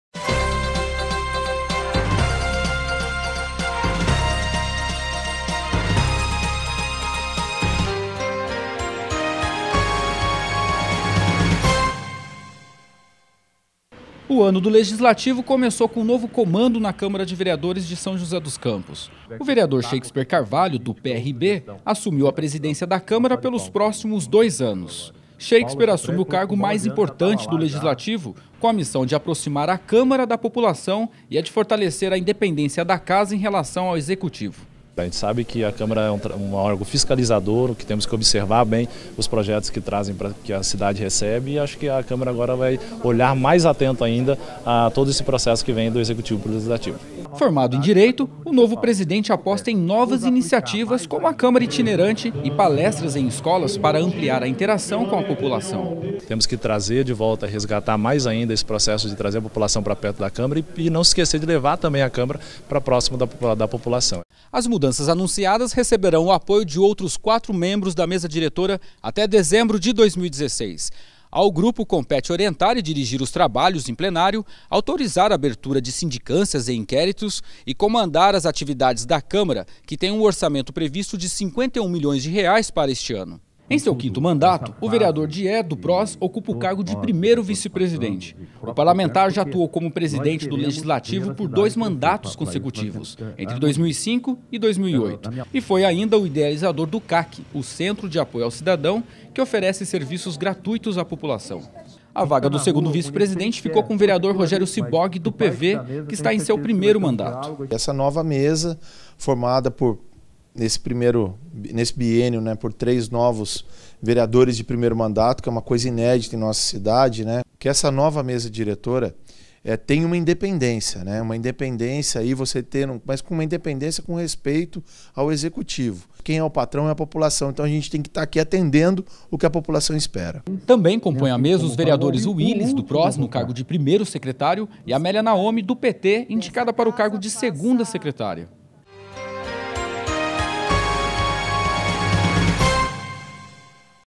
Atribuições da Mesa Diretora da Câmara de SJC
A Mesa Diretora da Câmara de São José dos Campos tem como objetivo orientar e dirigir os trabalhos em plenário, determinar a abertura de sindicâncias e inquéritos e comandar as atividades da Câmara. Além do Presidente, Shakespeare Carvalho (PRB), compõe a mesa: o vereador Dié, do PROS, que ocupa o cargo de primeiro vice-presidente, o vereador Rogério Cyborg, (PV) como segundo vice-presidente e os vereadores Willis (PROS) no cargo de primeiro secretário e Amélia Naomi (PT) para o cargo de segunda secretária.